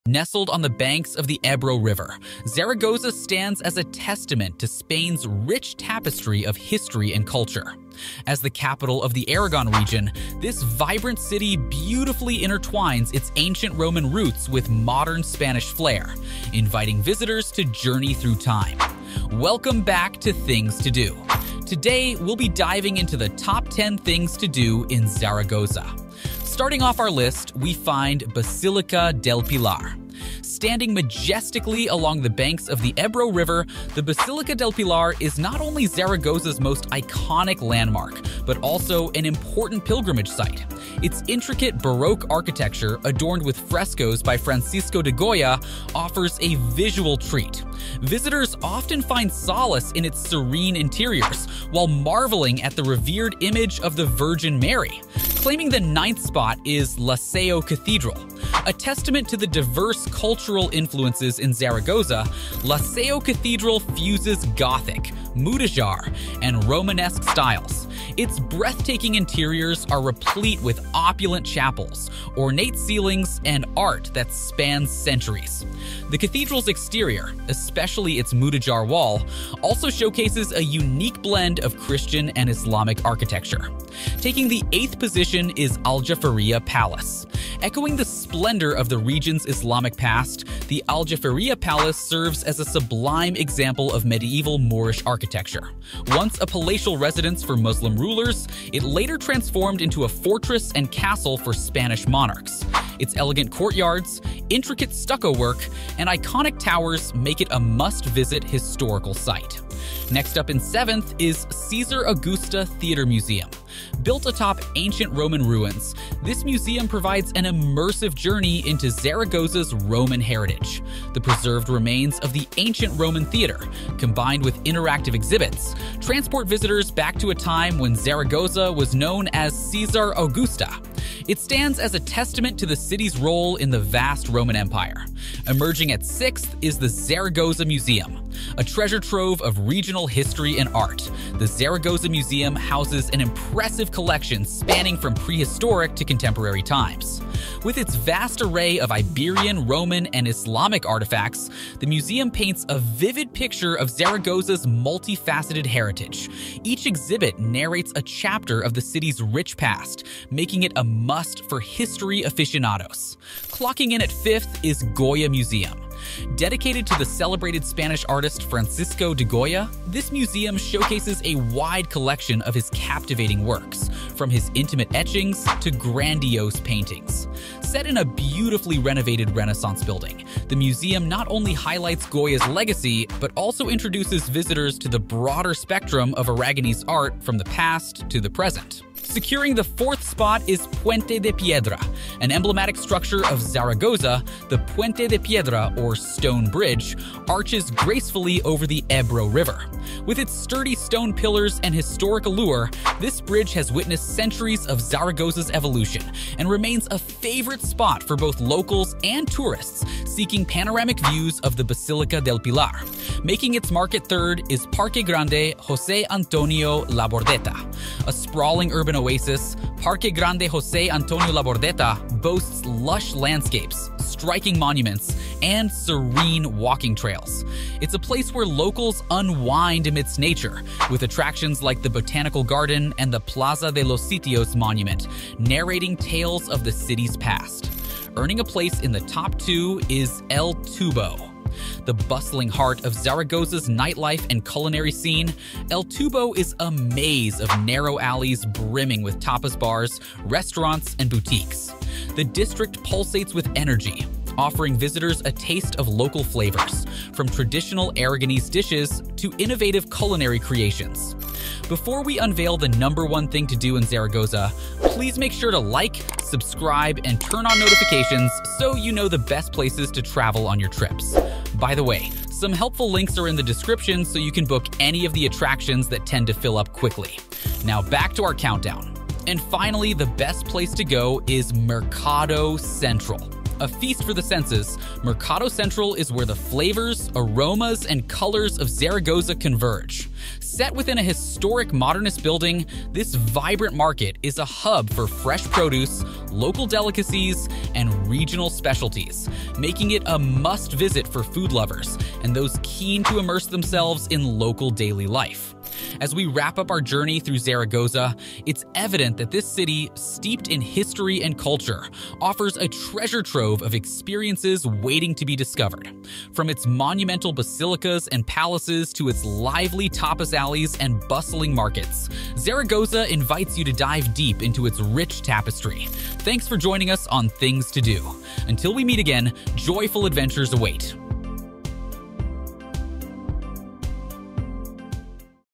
TOP 10 Things To Do In ZARAGOZA
This travel guide shares the best things to do in Zaragoza!
Nestled on the banks of the Ebro River, Zaragoza stands as a testament to Spain's rich tapestry of history and culture.

Standing majestically along the banks of the Ebro River, the Basilica del Pilar is not only Zaragoza's most iconic landmark but also an important pilgrimage site.

 A testament to the diverse cultural influences in Zaragoza, La Seo Cathedral fuses Gothic, Mudejar, and Romanesque styles.

 Echoing the splendor of the region's Islamic past, the Aljafería Palace serves as a sublime example of medieval Moorish architecture.

 Built atop ancient Roman ruins, this museum provides an immersive journey into Zaragoza's Roman heritage.

 A treasure trove of regional history and art, the Zaragoza Museum houses an impressive collection spanning from prehistoric to contemporary times.

Dedicated to the celebrated Spanish artist Francisco de Goya, this museum showcases a wide collection of his captivating works, from his intimate etchings to grandiose paintings.

An emblematic structure of Zaragoza, the Puente de Piedra, or "Stone Bridge", arches gracefully over the Ebro River.

A sprawling urban oasis, Parque Grande José Antonio Labordeta boasts lush landscapes, striking monuments, and serene walking trails.

The bustling heart of Zaragoza's nightlife and culinary scene, El Tubo is a maze of narrow alleys brimming with tapas bars, restaurants, and boutiques.

A feast for the senses, Mercado Central is where the flavors, aromas, and colors of Zaragoza converge.